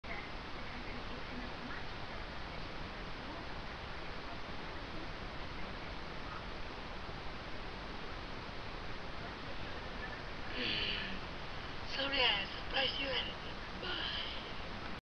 Choo Khay Senn Pop Up (NON SCARY)
Look carefully. Is only me doing a pop up. Don't be scared. I am not a ghost, I am human.